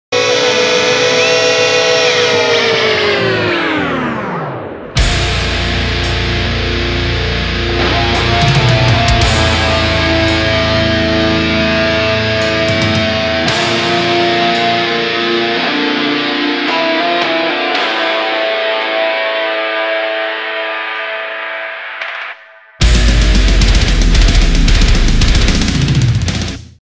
read description
soo my montage is like done, and I was talking to friends, and they said they would rather see monthly montages, than a montage were I just get my best clips possible over the course of 5 months which I just did.

Soo, what would you rather see? Monthly montages? or 1 good one every 5 months? decide for meeee